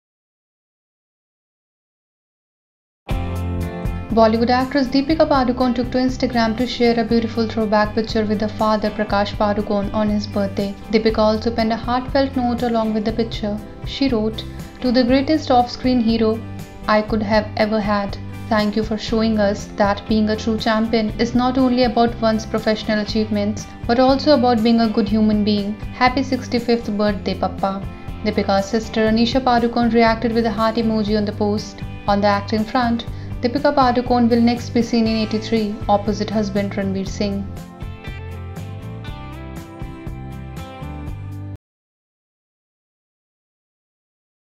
Deepika Padukone pens an emotional message for her father on his birthday
Bollywood actress Deepika Padukone took to Instagram to share a beautiful throwback picture with her father, Prakash Padukone on his birthday.